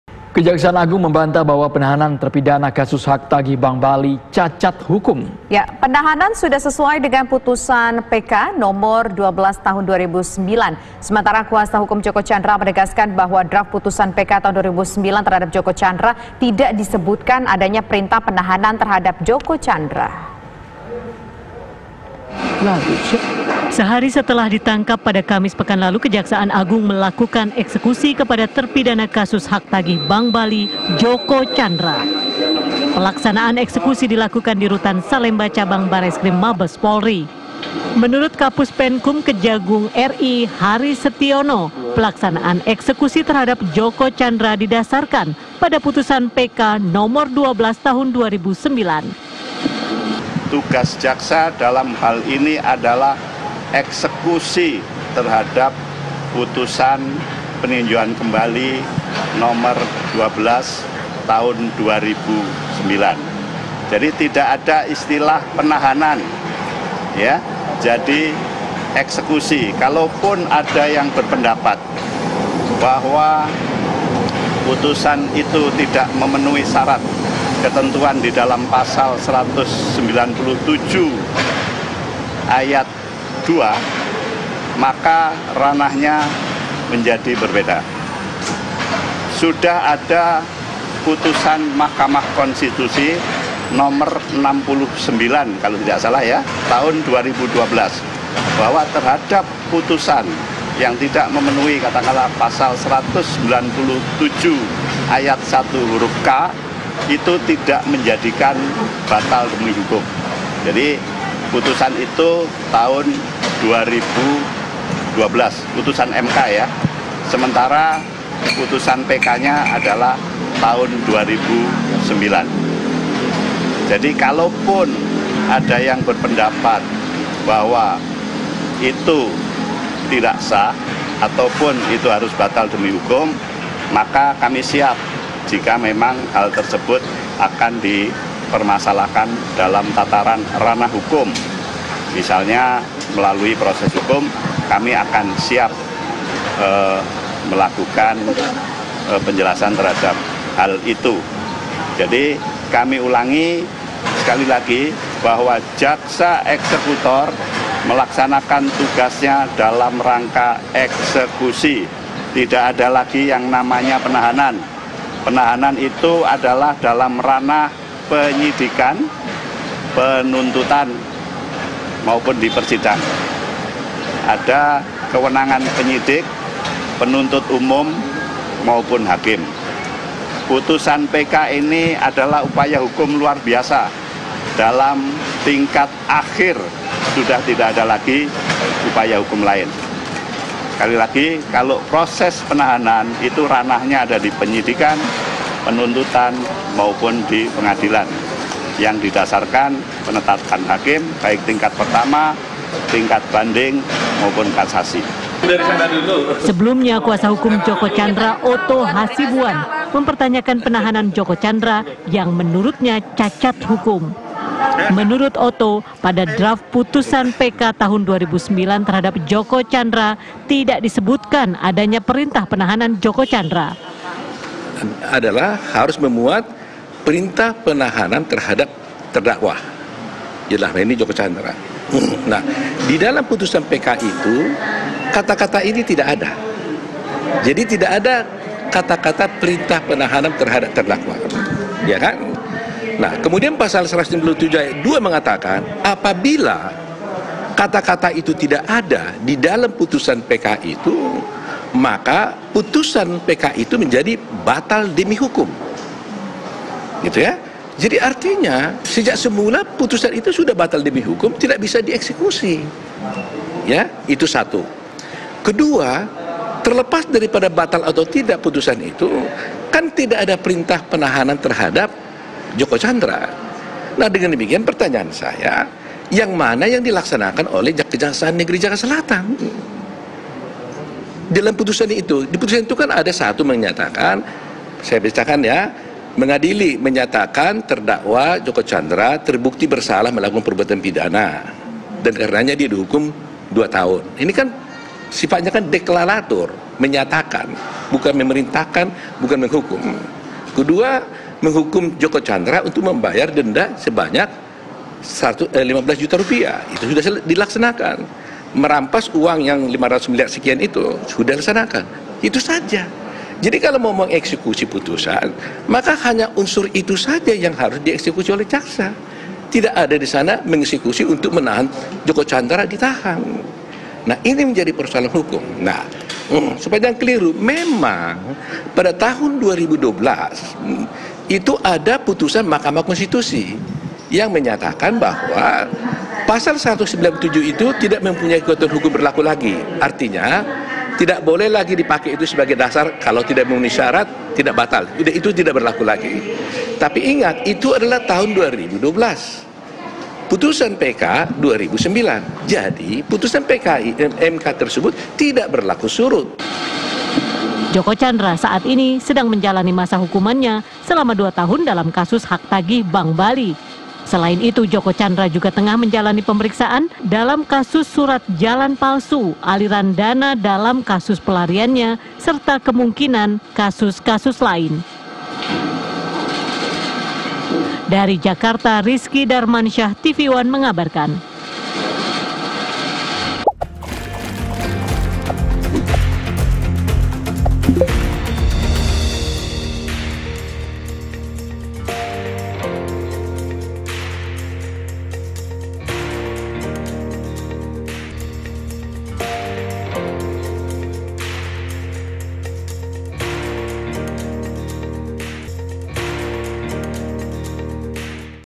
Dinilai Cacat Hukum, Kejagung Siap Hadapi Pengacara Djoko Tjandra | tvOne
Jakarta, tvOnenews.com - Kejaksaan Agung (Kejagung) menyatakan siap mengklarifikasi jika pengacara Djoko Tjandra, Otto Hasibuan menempuh halur hukum terkait status penahanan terhadap kliennya. Sebab, penahanan terhadap terpidana kasus hak tagih Bank Bali itu berdasarkan putusan Peninjauan Kembali (PK) Mahkamah Agung